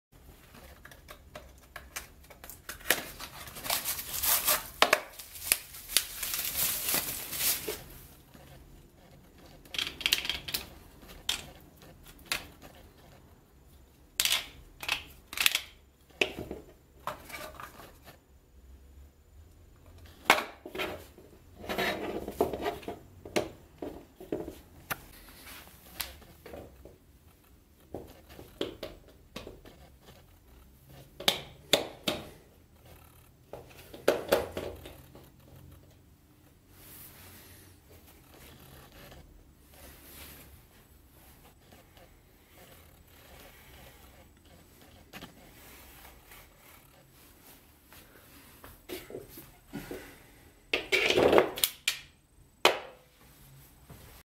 360 Degree Rotating Foldable Stand Holder for Tablet PC Cellphone
This foldable desktop stand is rugged and stable. My iPad fits perfectly but you can use it as a smartphone or book stand too. It is only 90g and folded about 180 x 32 x 26mm small. The ABS plastic and the joints seem sturdy to me. You can get it for around 4$